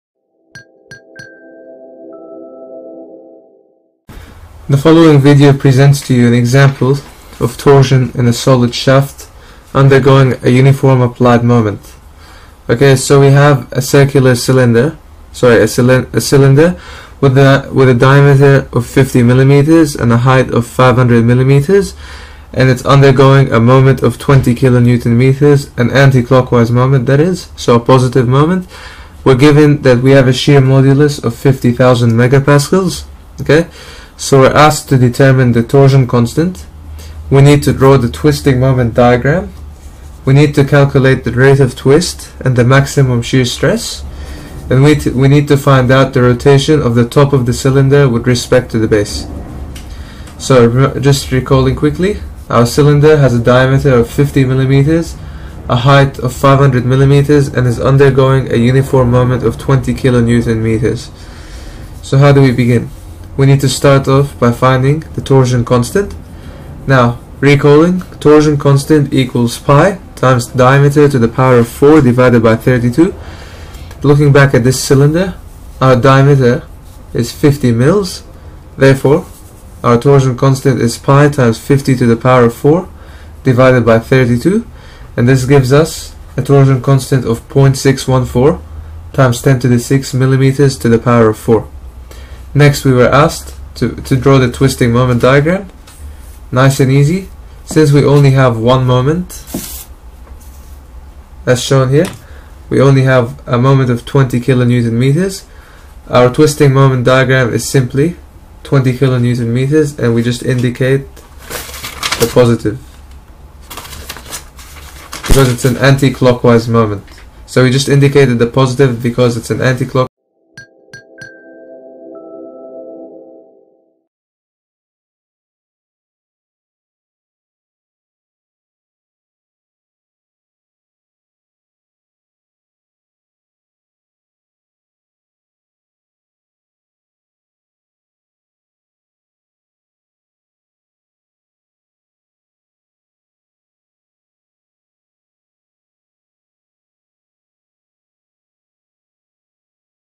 Uniform Torque on a Cylindrical Shaft Example | Mechanics of Solids
for more FREE video tutorials covering Mechanics of Solids and Structural Mechanics

This video presents an example of torsion of solid shaft undergoing a uniform twisting moment. At first, the video illustrates the given exemplary cylindrical section having diameter of 50 mm, height of 500 mm and an anticlockwise moment of 20 KNm. The shear modulus of the cylindrical shaft has also given. The example asks to calculate the torsion constant, rate of twist, maximum shear stress, and number of rotation of the top cylinder with respect to base as well as asks to draw the twisting moment diagram.

Moving on, the video shows how to determine the torsion constant from the given data and formula described in previous video followed by the drawing of twisting moment diagram. Later, the video demonstrates the entire procedures to calculate rate of twist and maximum shear stress in details. The video finds that maximum shear stress occurs at the maximum radius away from the center of the circular cross section. Finally, the video shows how to find out the number of rotation of the top cylinder with respect to base.